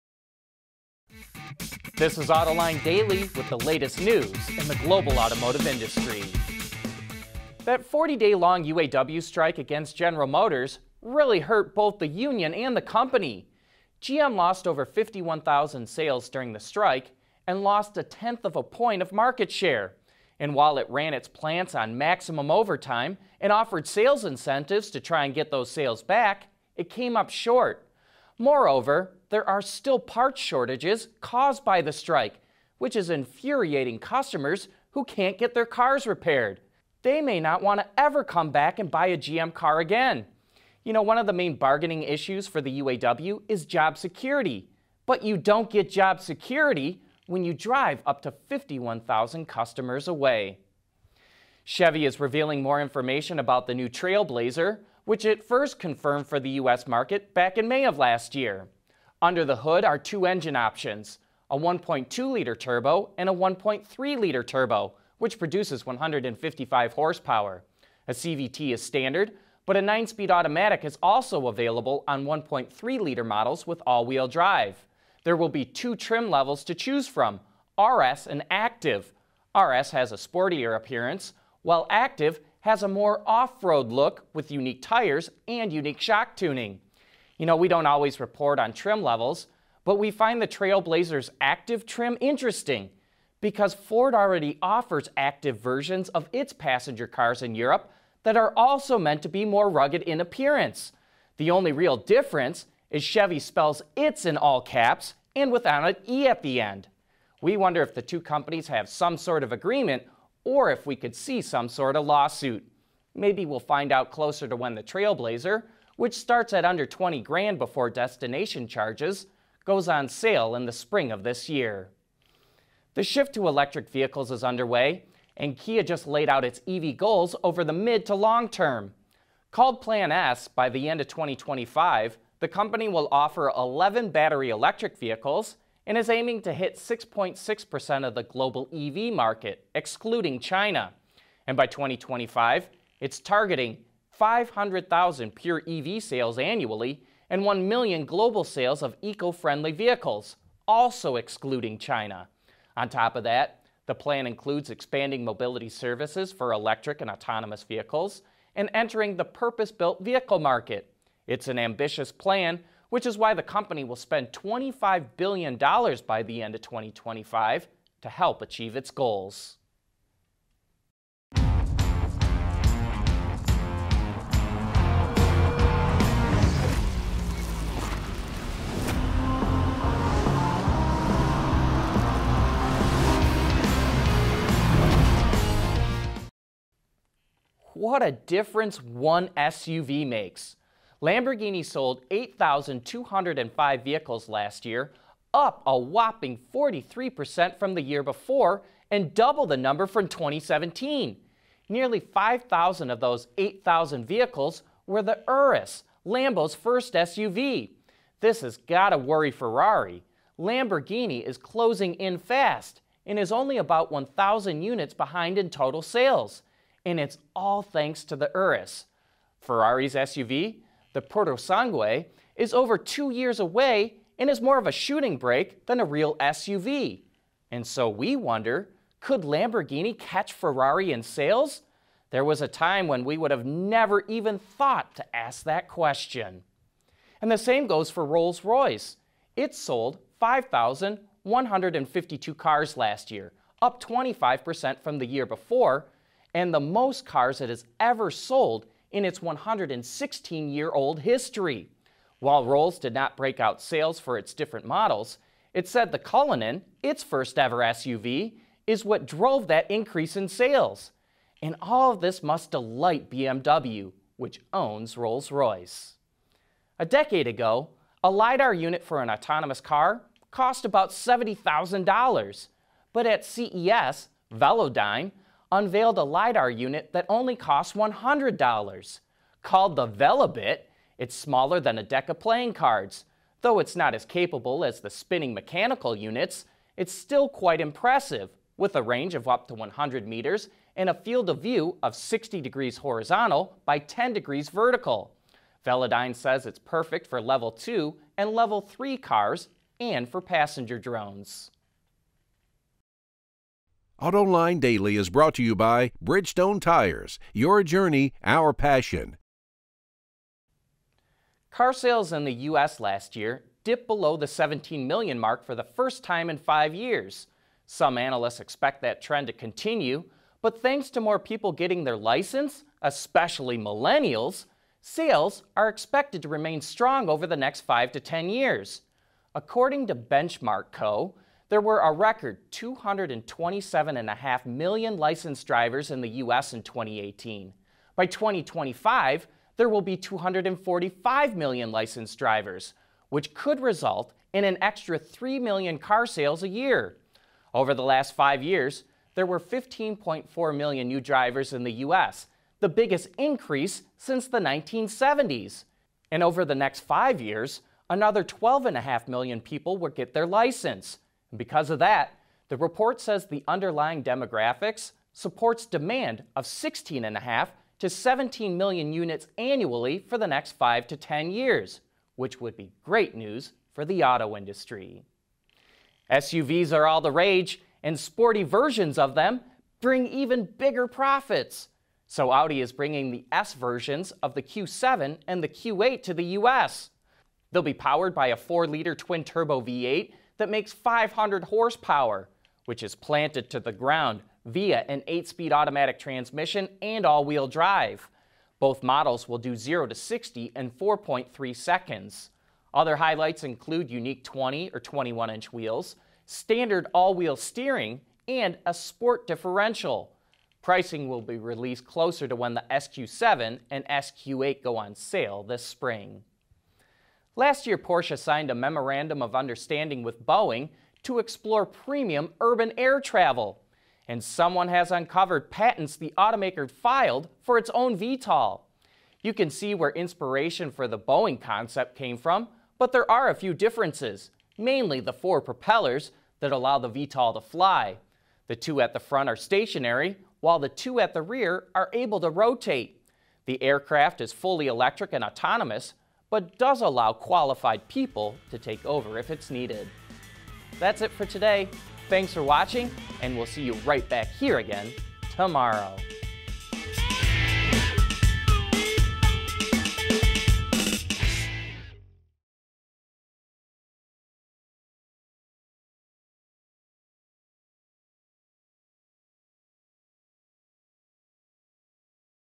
New Trailblazer Details; Indication Car Sales Stay Strong - Autoline Daily 2750
0:07 Strike Hurt GM & UAW
0:51 New Chevy Trailblazer Details
2:02 Kia Plans for Its Electric Future
3:07 Urus Drives Huge Growth for Lamborghini
3:57 Rolls-Royce Has Its Best Sales Year Ever
4:26 Velodyne Develops Affordable Lidar
5:10 Car Sales Will Remain Strong for 5 to 10 Years
6:15 Audi Bringing SQ7 & SQ8 to the U.S.
6:56 Porsche Files Patents for VTOL